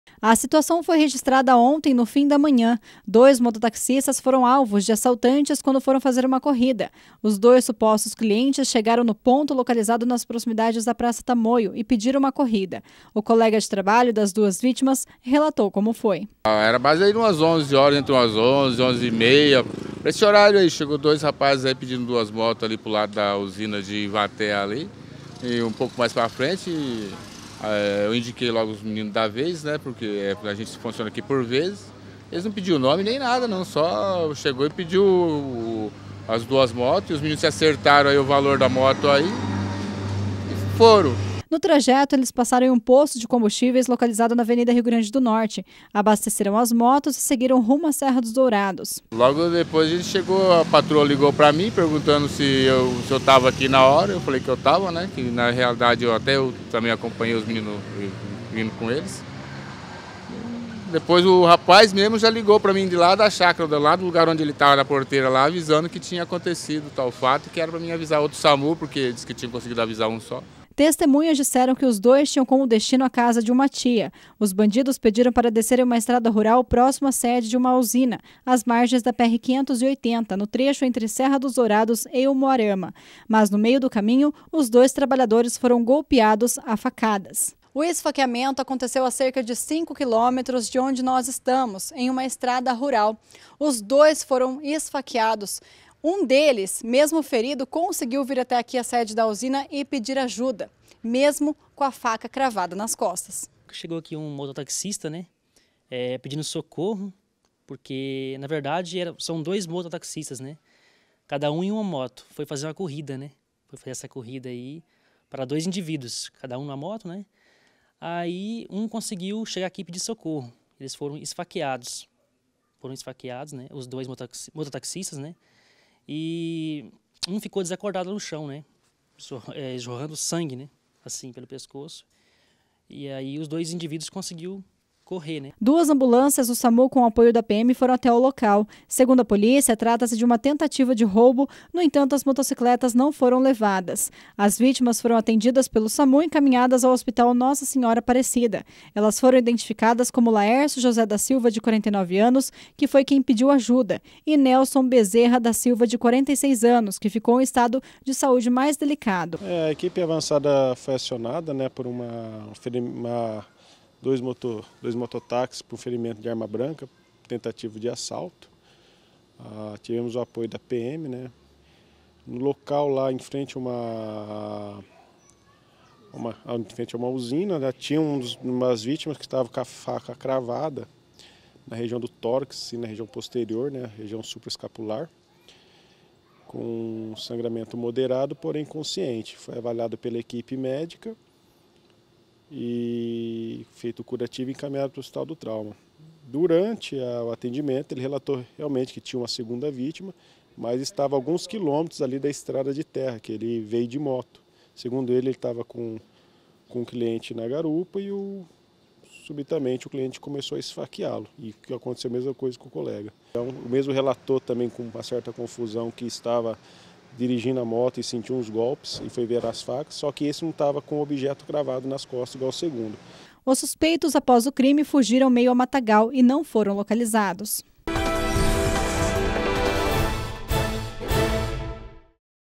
Mototaxistas ficam gravemente feridos após serem esfaqueados em Umuarama
Os mototaxistas foram esfaqueados por dois rapazes que teriam se passado por clientes.